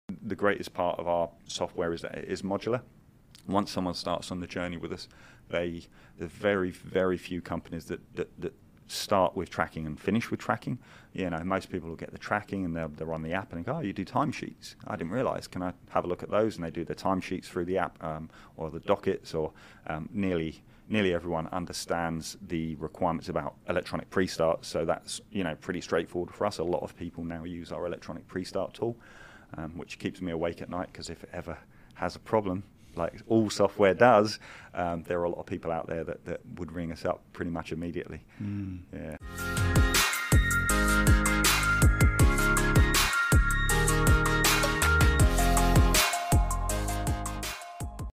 Jim Lee - mentions that making the software modular is its greatest feature
Jim Lee of The Fleet Office mentions that making the software modular is its greatest feature, allowing almost everyone to start and end tracking while using its timesheets and electronic prestart tool.